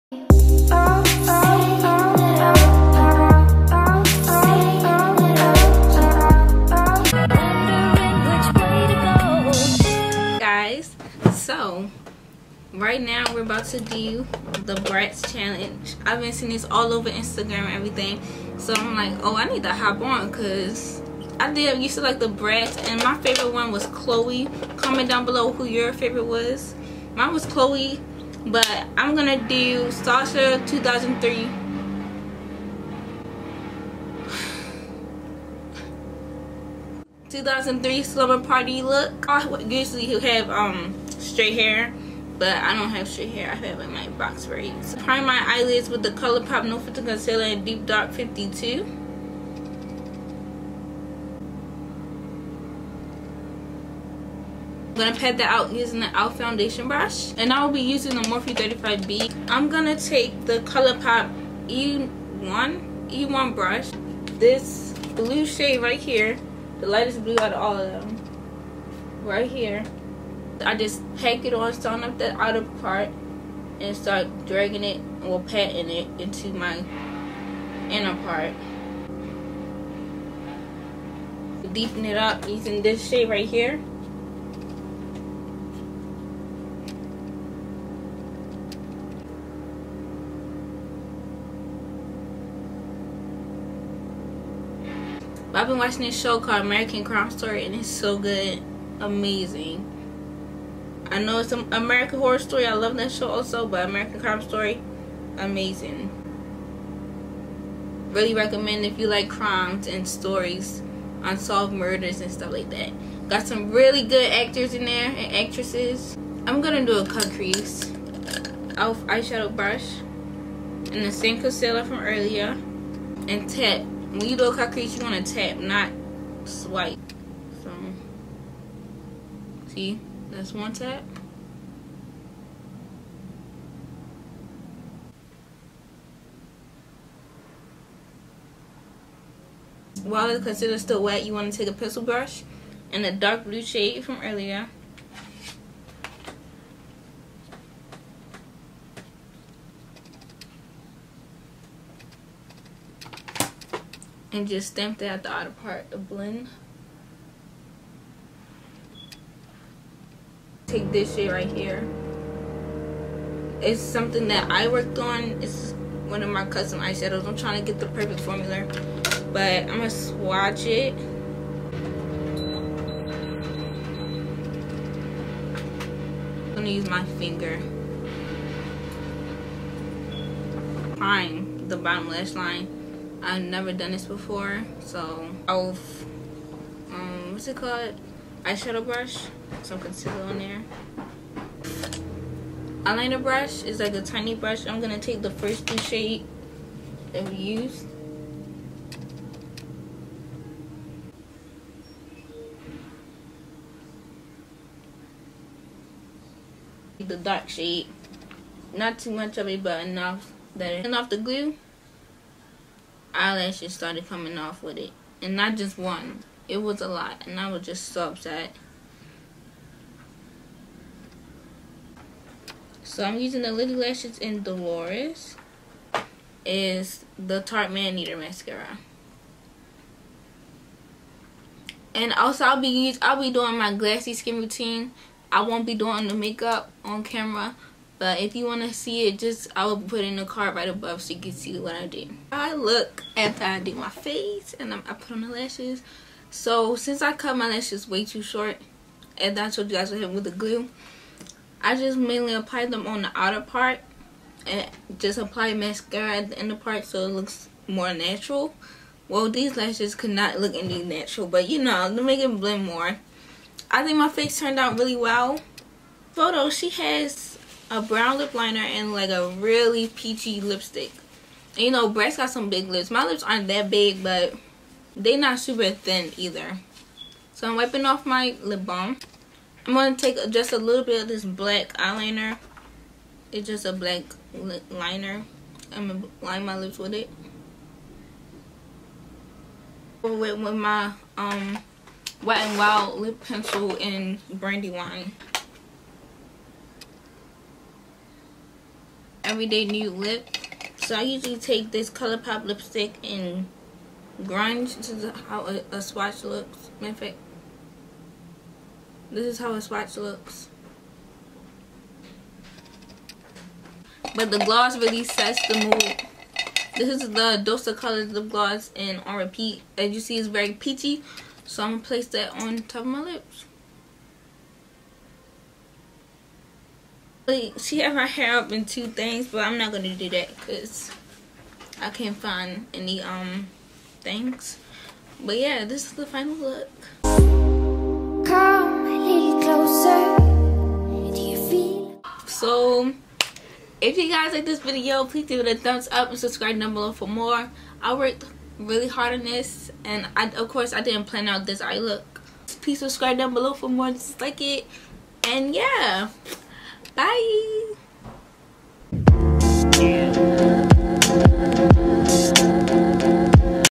Sasha Bratz Challenge Inspired Makeup | Jo Beauty
Hey guys so today I did a bratzchallenge look. I did sasha .